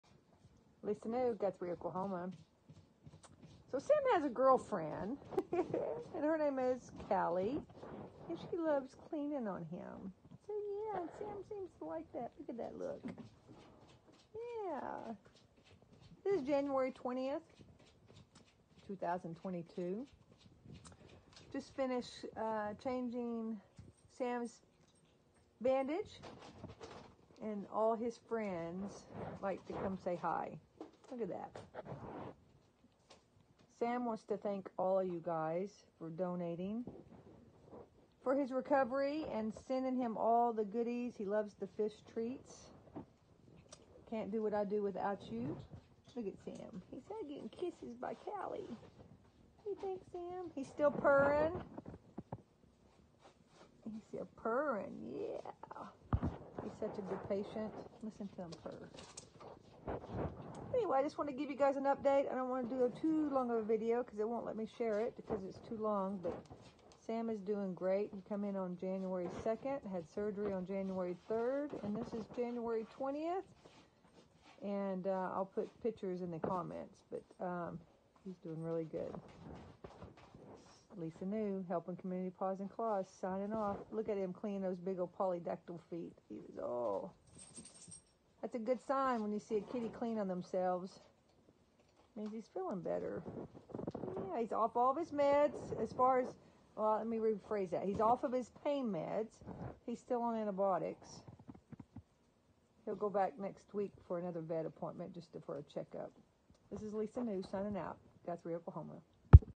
Cat Severely Abused During Halloween Was Finally Trapped And Taken To The ER
We got a call about Sam in Oct. 2021, the photo of his condition was shocking. We couldn’t believe what we were looking at and that he was still alive. Half of Sam's body was flesh and infection. You could smell Sam's infection from across the street. It is believed that he was tortured by horrible people in the neighborhood during Halloween and now he wouldn't come near a human- even if they were a helping hand.

For two months we desperately were trying to trap him so we could help his urgent medical needs. The weather was becoming unbearable for poor Sam as it was freezing outside. He found shelter in a garage and that's when we rushed over to catch him.

The house owner told us he has been suffering on the street for almost a year. This open infected wound has taken over most of his body and poor Sam must be in agony. It’s going to be a long recovery but we are so thankful to have gotten him off the streets before his injuries took his life. He is being taken to the vet every two days for a bandage change and for more medications. It will take months for the wound to get smaller and then we will move forward with surgery to close it if needed.

Despite everything he has been through and all of the pain abusers put him through, he is the sweetest boy. We are showing him that humans can be kind. There is no doubt that this boy is a fighter- we are confident he will make a speedy recovery with his will to live.